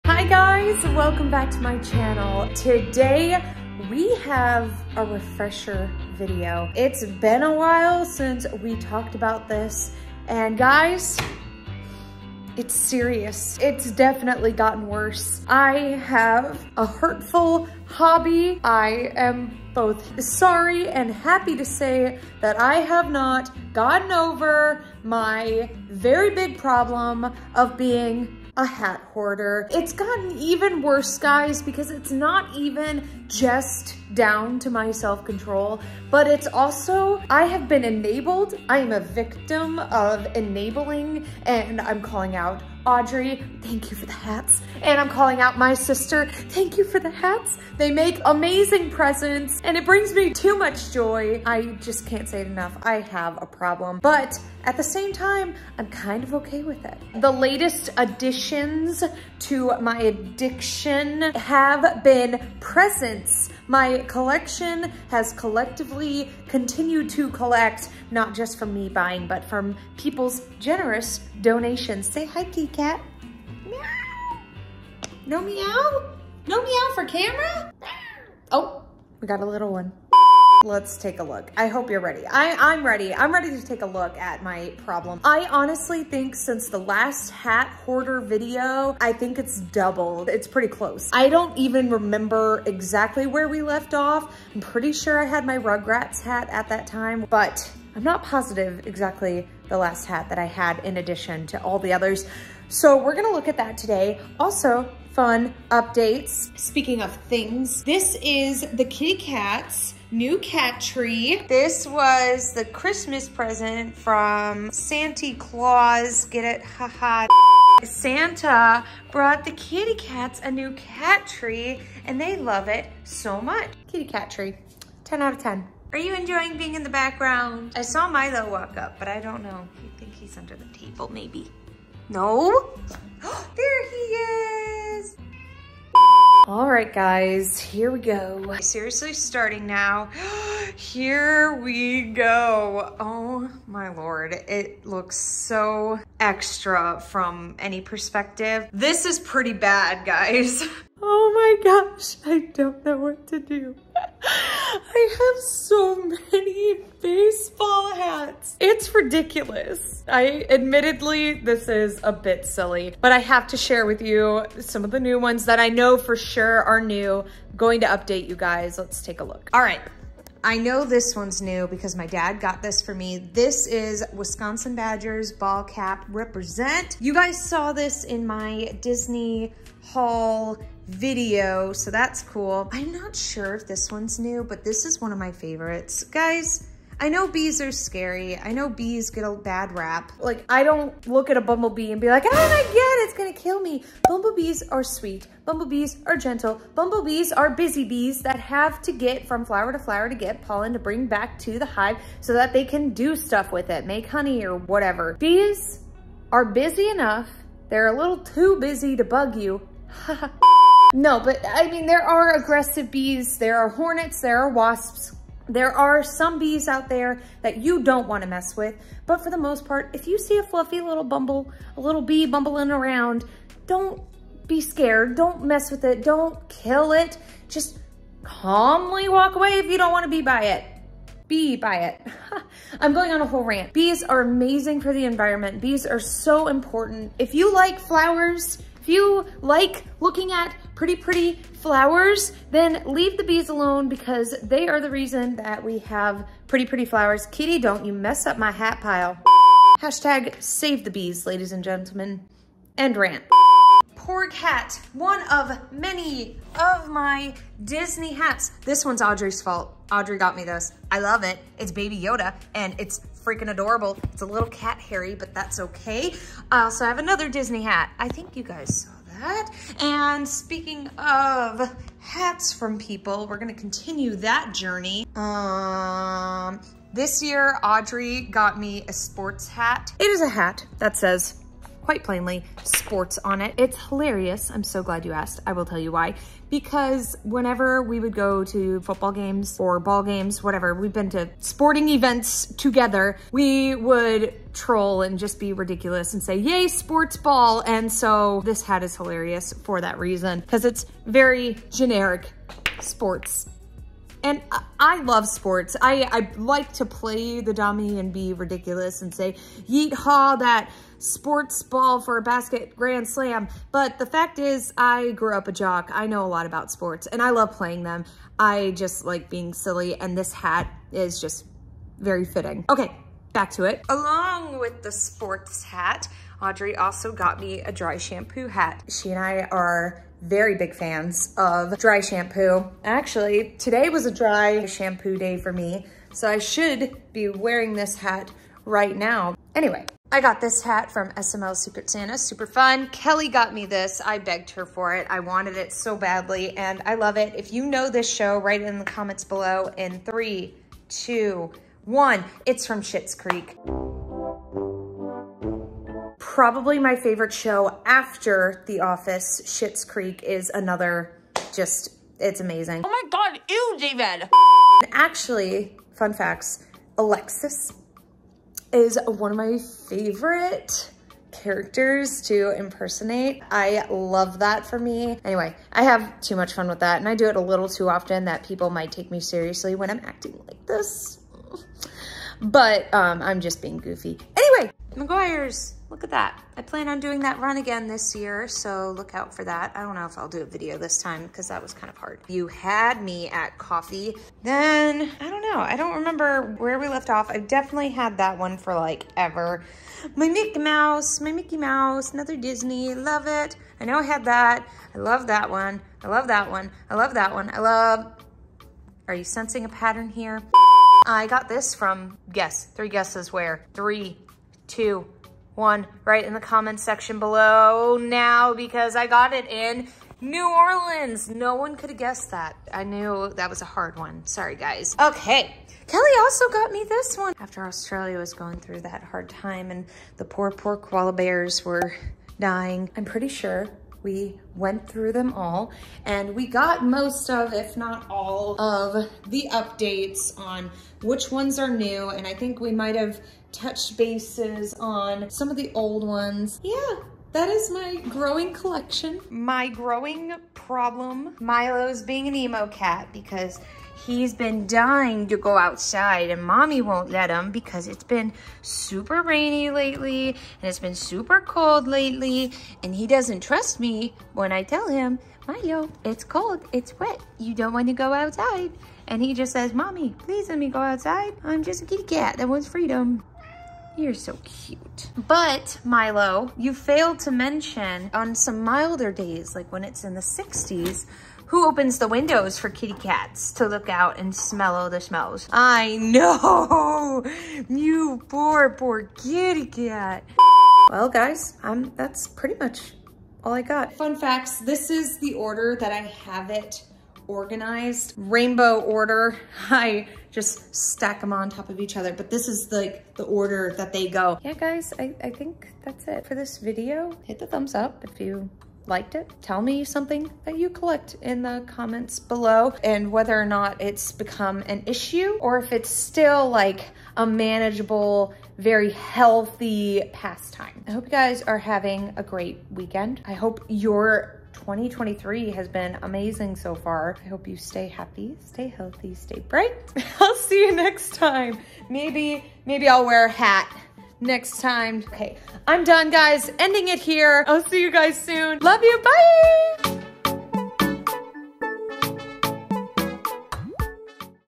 It Has Gotten WORSE!! (Hat Collection Vlog)
Hi guys! I hope you enjoy this vlog! All jokes aside- I love and am very proud of my collection! I hope you enjoy this glimpse into my realm of hats lol